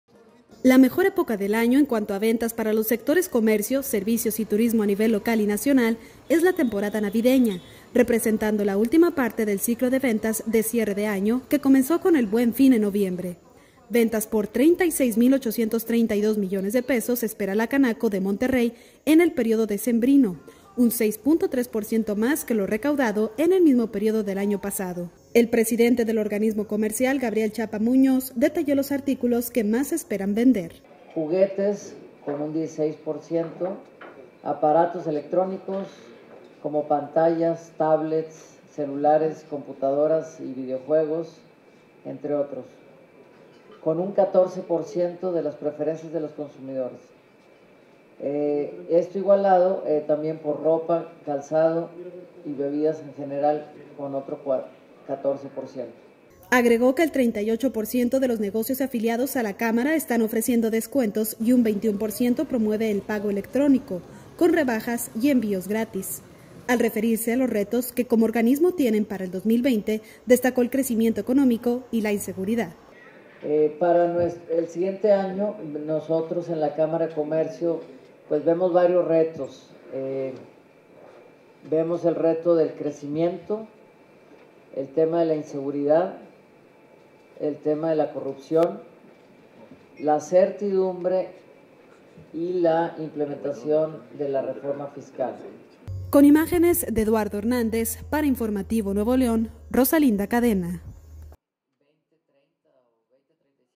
Espera Canaco Monterrey mayores ventas en periodo decembrino
Espera Canaco Monterrey ventas por más de 36 mil millones de pesos en este periodo decembrino.